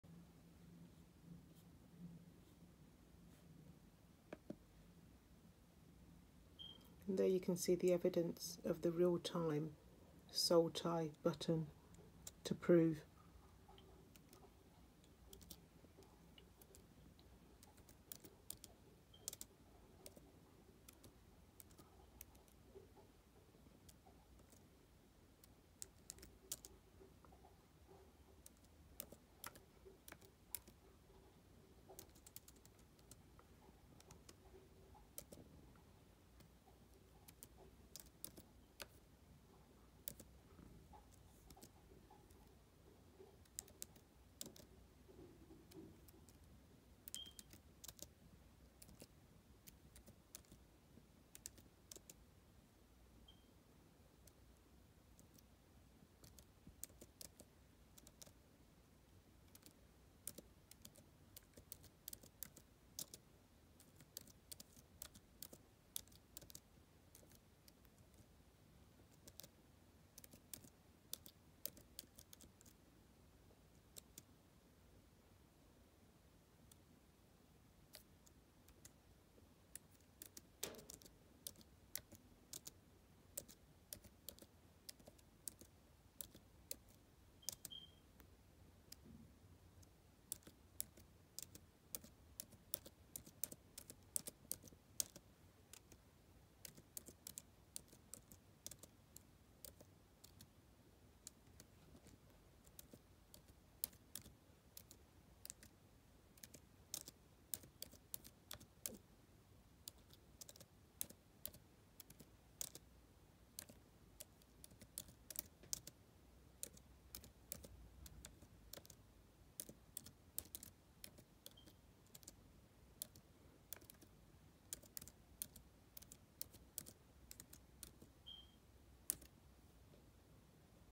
GERMAN SHOOTING
REAL TIME SOUL TIE HACKING PROOF SHOOTINGS ARE ORGANISED BY THE MOB.GOV IN AN ATTEMPT TO STEAL MY IDENTITY ACCESS TO ATLANTIS JAMAICA.

EVIDENCE AS I TYPE - LIGHT BLUE SILICON VALLEY DNA ENERGY HARVESTING BUTTON APPEARS COURTESY OF SQUARE SPACE.

NO CONSENT NO CONTRACT - ABSOLUTELY NOT